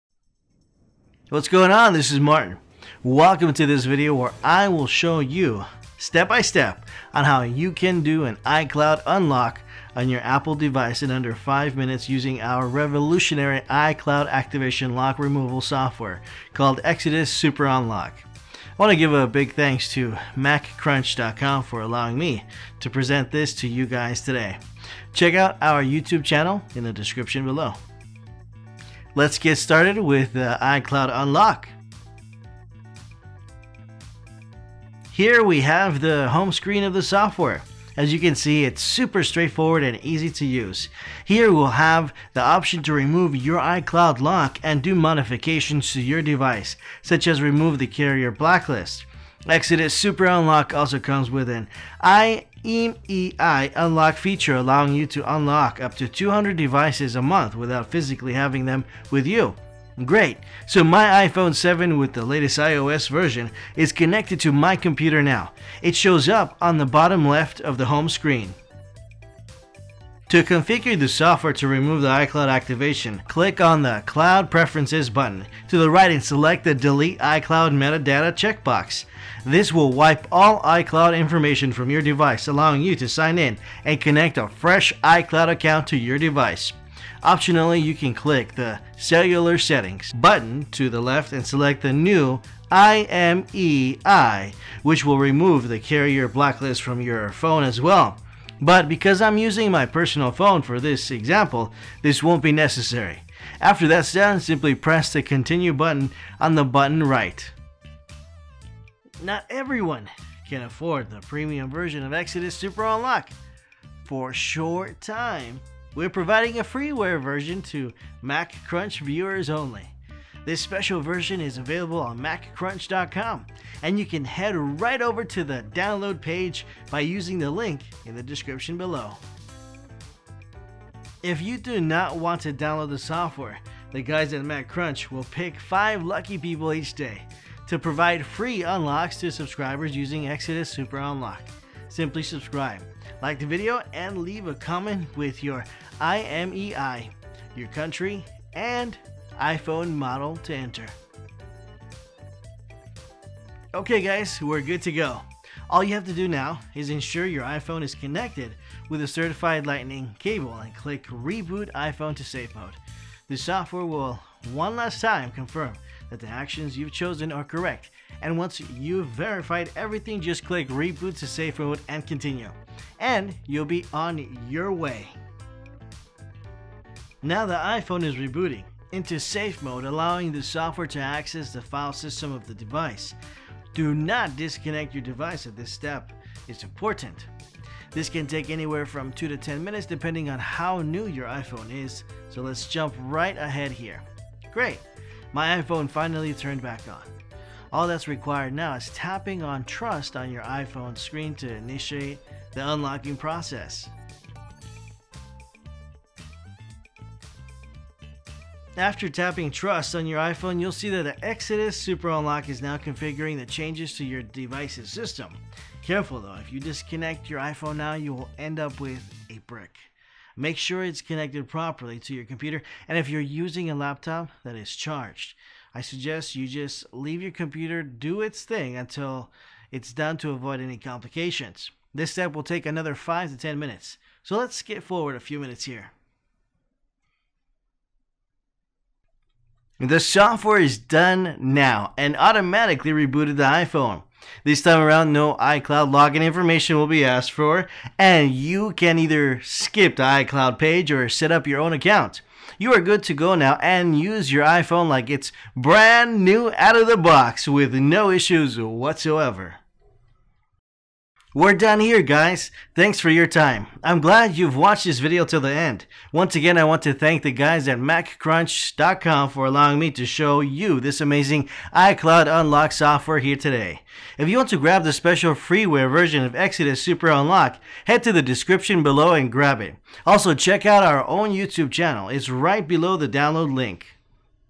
How to Remove iCloud Lock from an iPhone 6
In this video we show how to remove iCloud Lock from an iPhone 6 using Exodus Super Unlock. You can find the software on ExodusSuperUnlock.com

For any questions comment in the description below